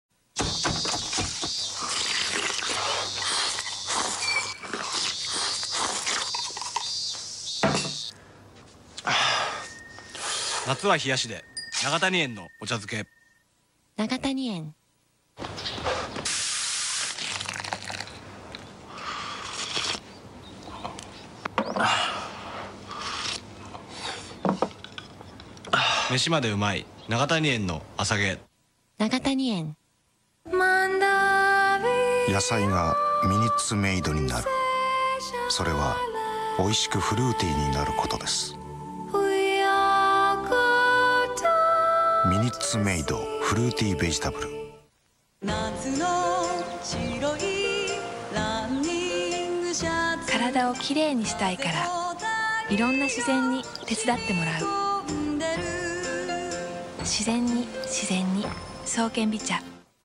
懐かしいCM（2003年）#0016 (Japanese Commercials)
永谷園 NAGATANIEN「夏は冷やしで。永谷園のお茶づけ海苔」
永谷園 NAGATANIEN「めしまでうまい。永谷園のあさげ」
日本コカ・コーラ Coca-Cola「MinuteMaid」
日本コカ・コーラ Coca-Cola「爽健美茶」